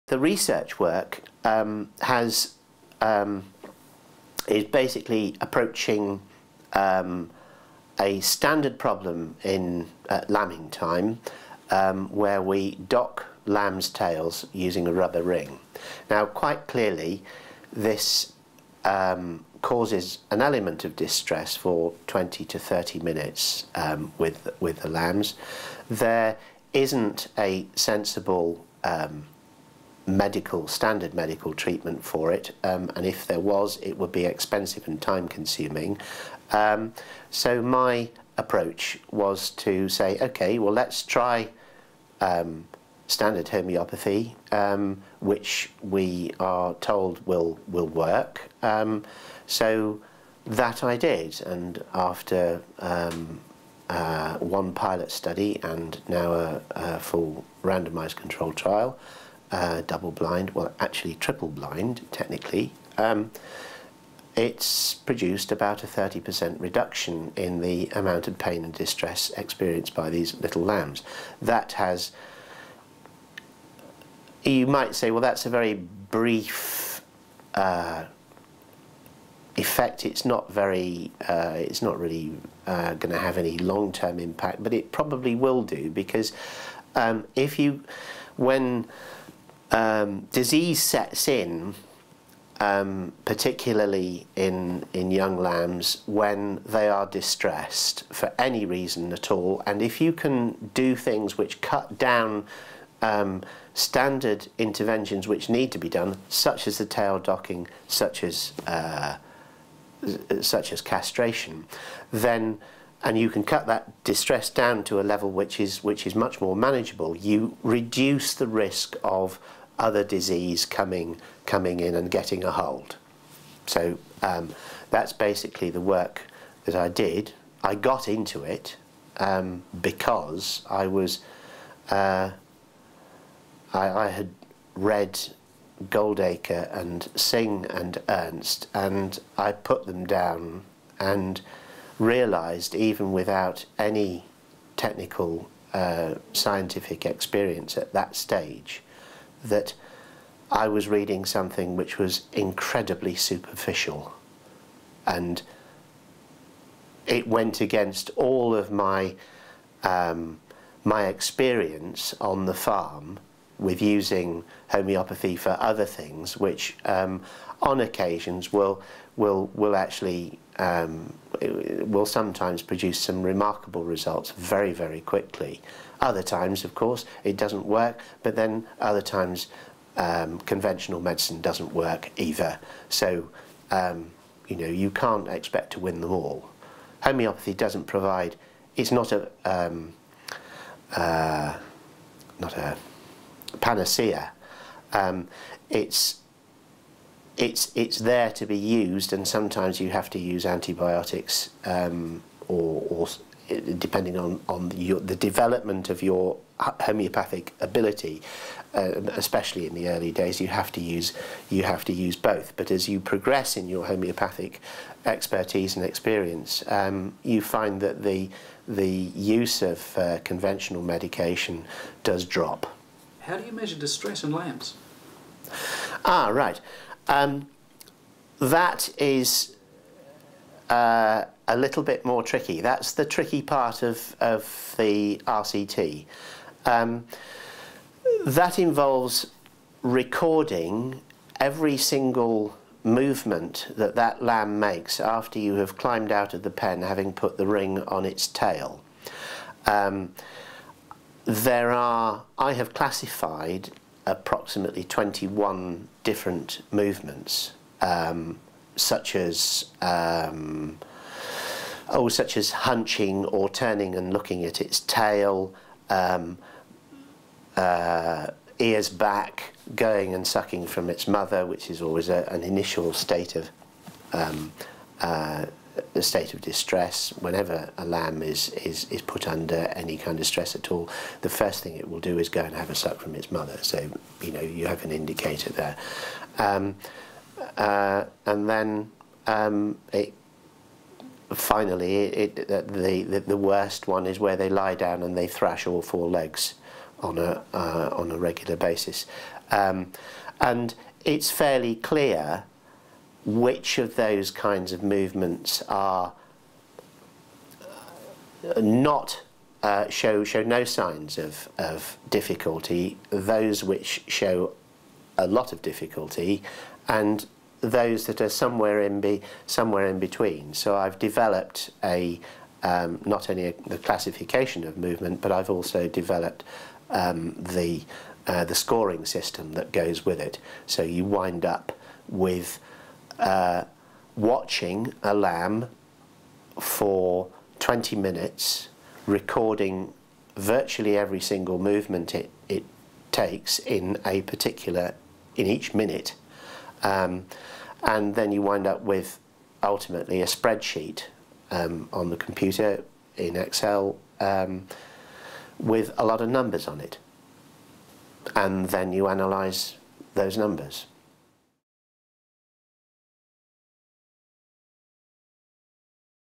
David Eyles | Triple blind RCT showing homeopathy reduces distress in tail-ringed lambs
Farmer David Eyles on his research, the impact of reading Ben Goldacre's book and how he uses both conventional medicine and homeopathy on his farm.